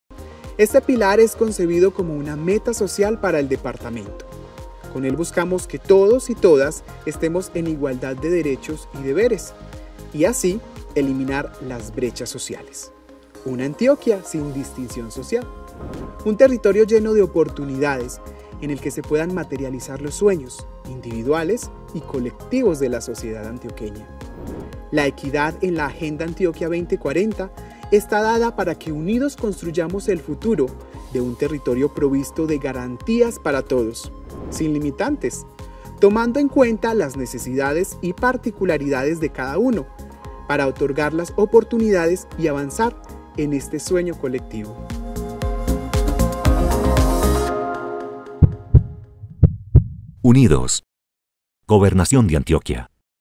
Antioquia2040 | Pilar de Equidad
Trabajamos en el marco de cinco pilares para la deliberación ✅

El pilar de 𝗘𝗾𝘂𝗶𝗱𝗮𝗱 implica la capacidad de ser justos ⚖ considerando las necesidades individuales ☝🏼 y las circunstancias de cada persona 💭 en el territorio.